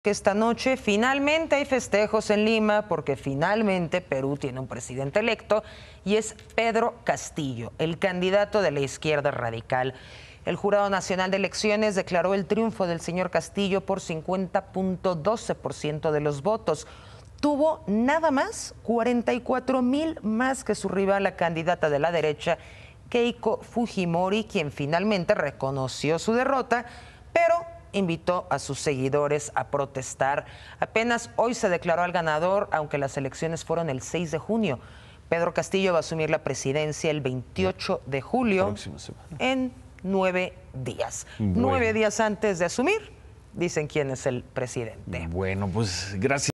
Pedro Castillo será el próximo presidente de Perú | Noticias con Ciro Gómez Leyva
Pedro Castillo será el próximo presidente de Perú, después de derrotar a Keiko Fujimori en las elecciones del 6 de junio.